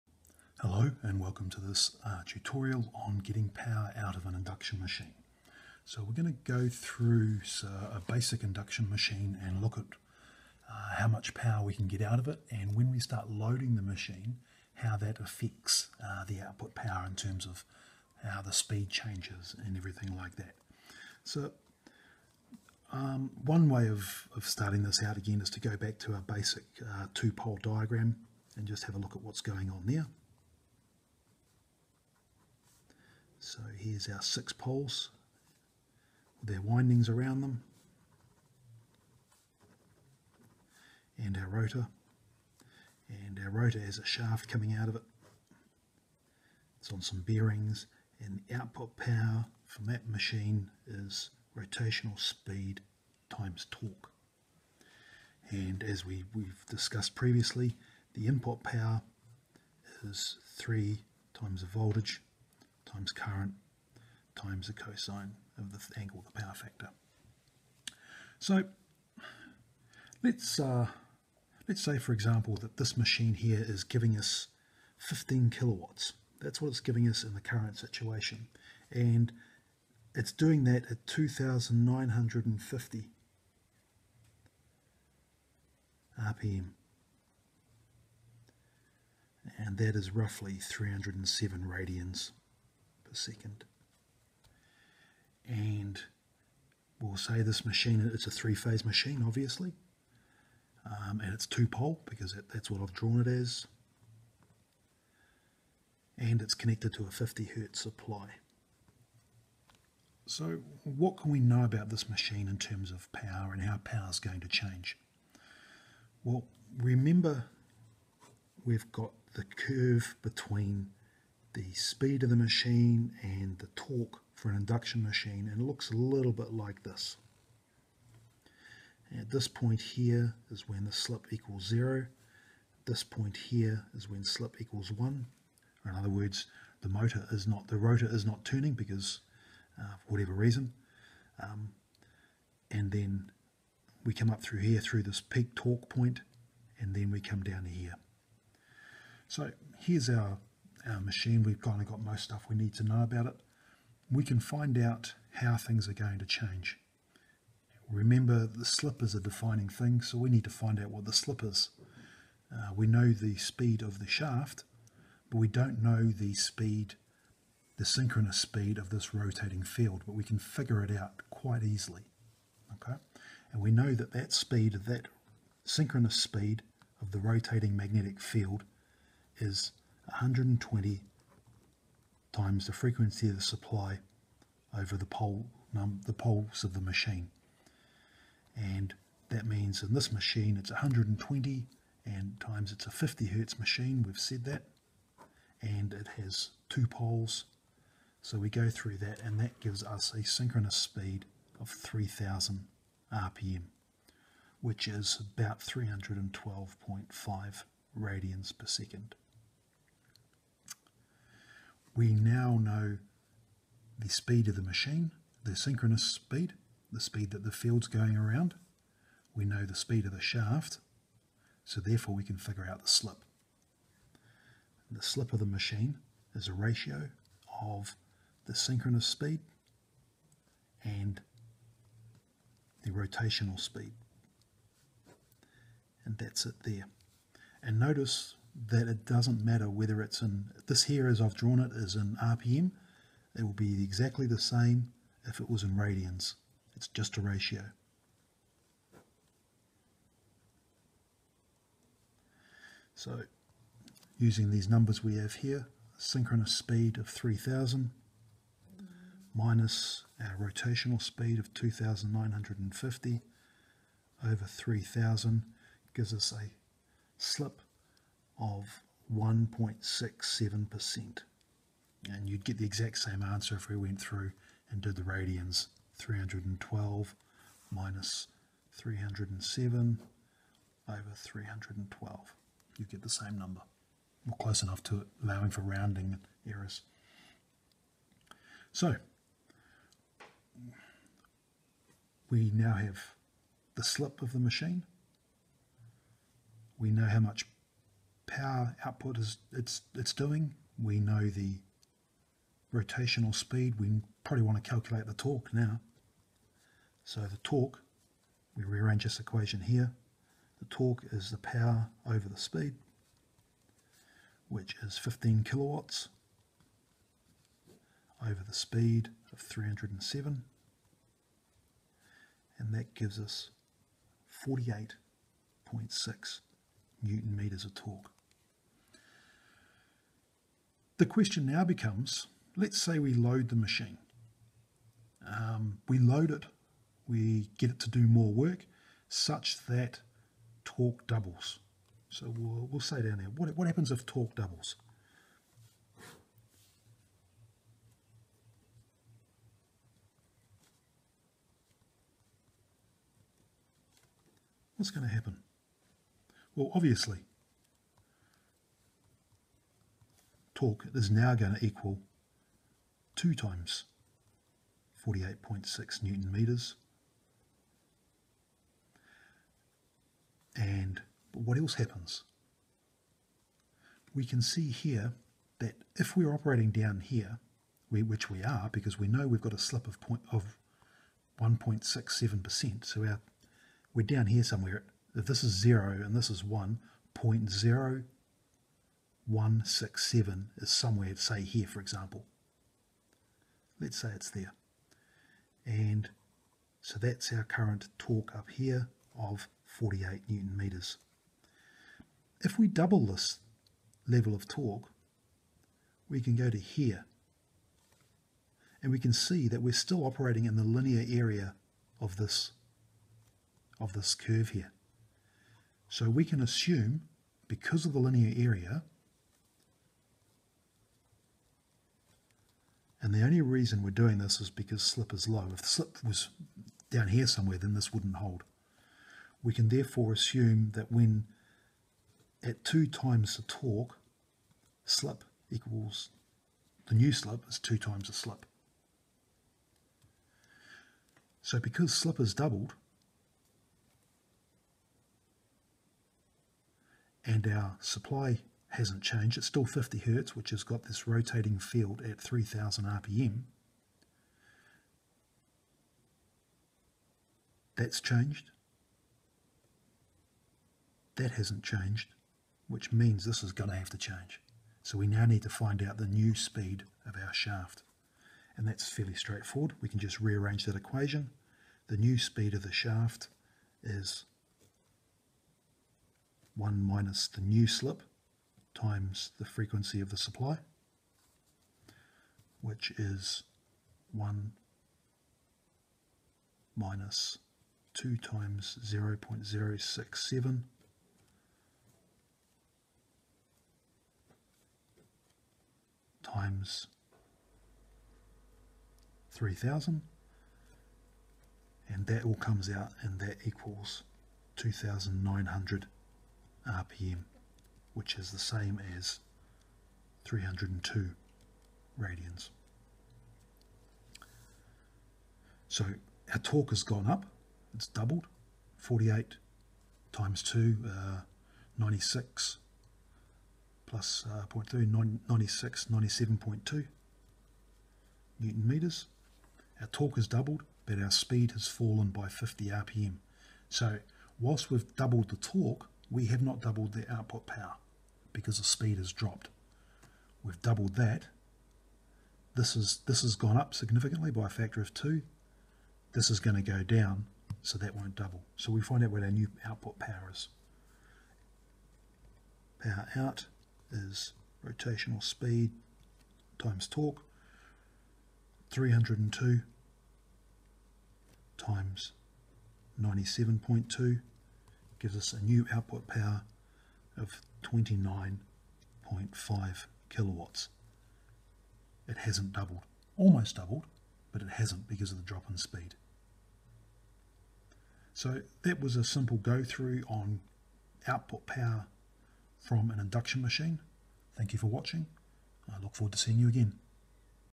Taylormade@AUT: Output power from an Induction motor
This tutorial looks at some simple calculations around an induction machine, and how how output speed can change when the machine is loaded.